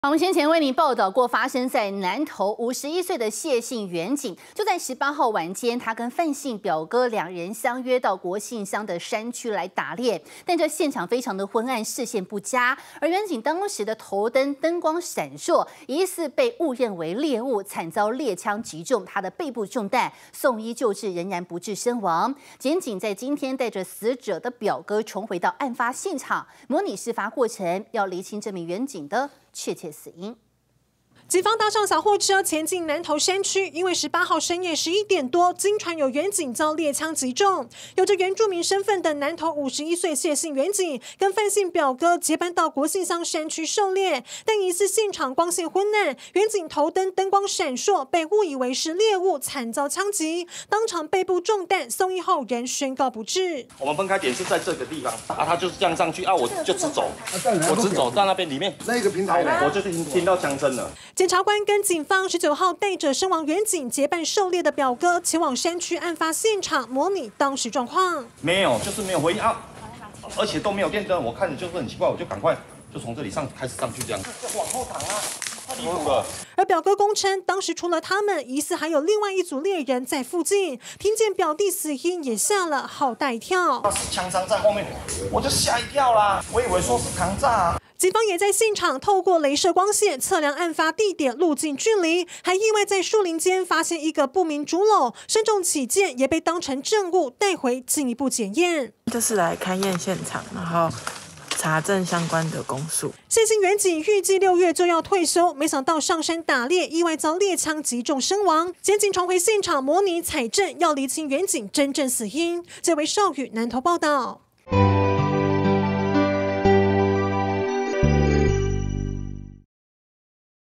詭！ 警深夜打獵遭槍擊中亡 檢警回案發地盼「釐清死因」
南投51歲謝姓員警，18日晚間跟范姓表哥相約到國姓鄉山區打獵，但現場昏暗視線不清，員警頭燈燈光閃爍，疑似被誤認為獵物，慘遭獵槍擊中，背部中彈送醫不治身亡，表哥供稱當時除了他們，疑似還有另組獵人在附近， 聽見表弟死因也嚇了好大一跳。檢警19日帶著死者表哥重回案發現場，模擬事發過程，要釐清員警的真正死因！